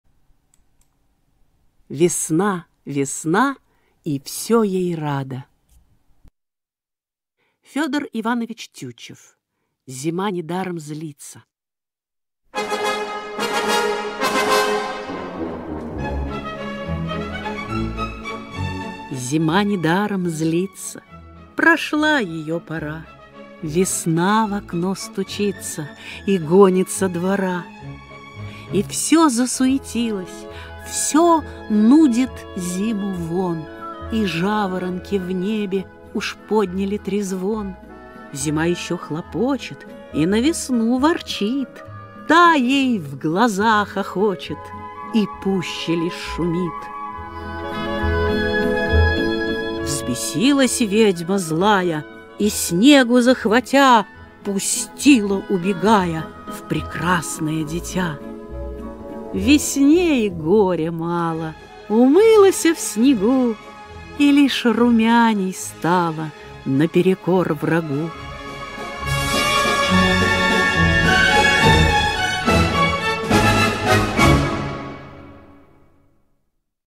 Ф. Тютчев "Весна"
Аудиоприложение к учебнику "Литературное чтение" 2 класс